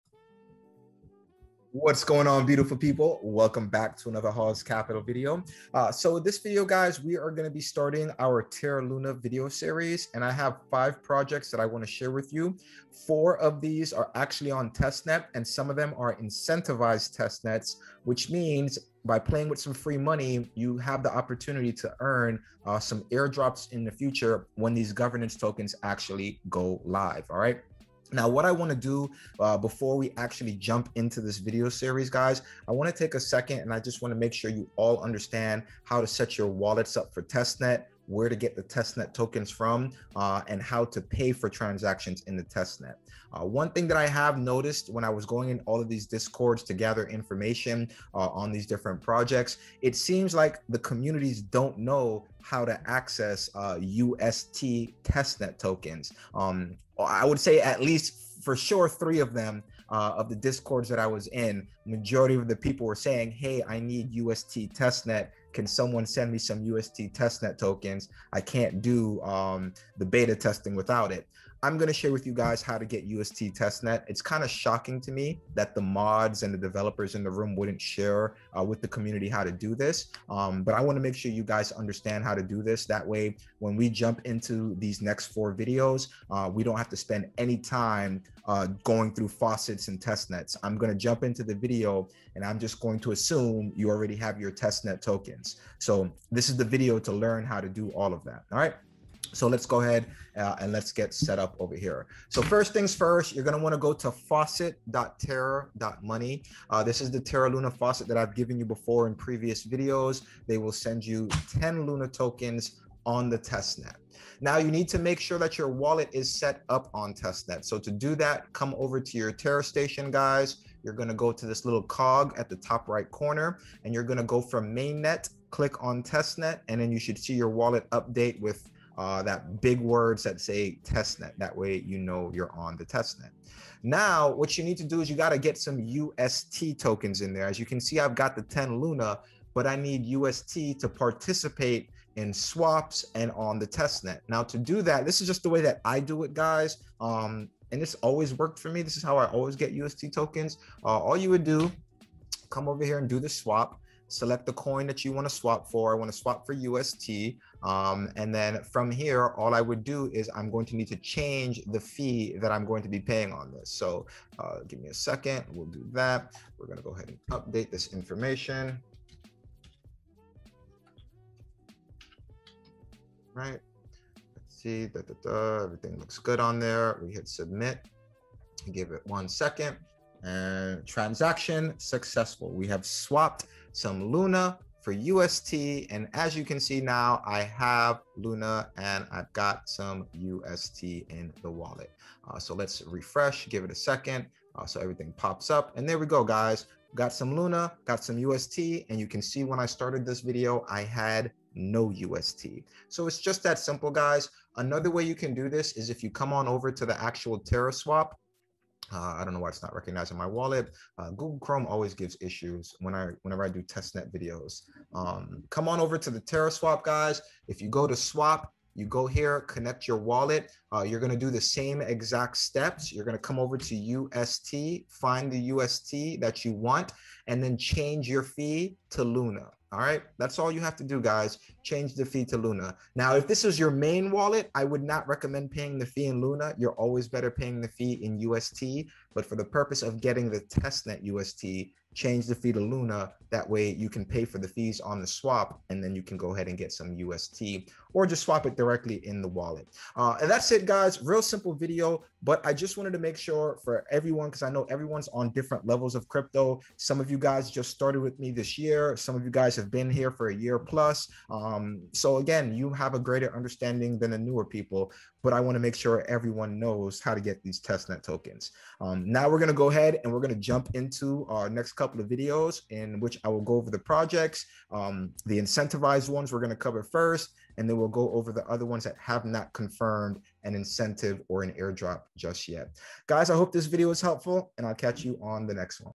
Full Terra Luna Testnet Guide. | Luna testnet wallet | How to get UST Testnet tokens and beta test.
After visiting a few project discords it come to my attention many people dont know how to fund their test net wallets and most people dont know how to get UST into the LUNA wallets so they can go play on testnet.
I will show u a simple trick i  use to get LUNA and UST into my test wallet.
Luna
Terra luna testnet
Ust testnet

** Nothing on this page constitutes Financial advice in any way or form all videos are created for entertainment use only**